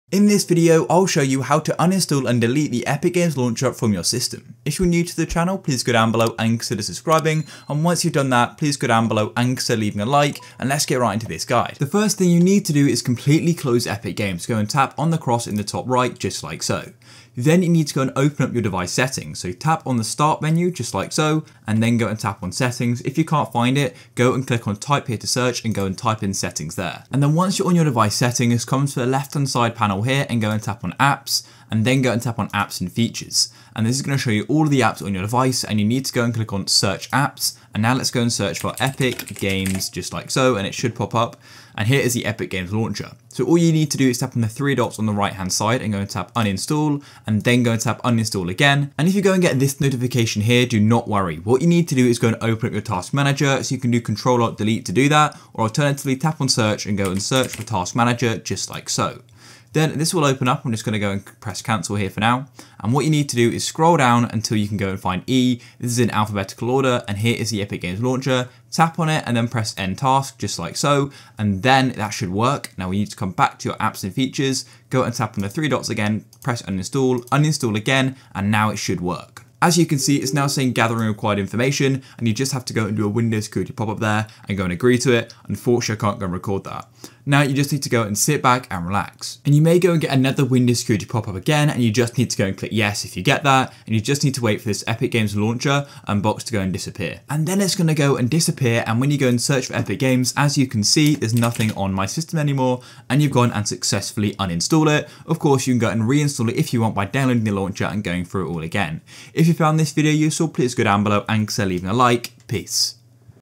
How To Delete Epic Games Launcher (Full Guide) - Uninstall Epic Games Launcher
I show you how to delete epic games launcher and how to uninstall epic games launcher in this video.

GuideRealm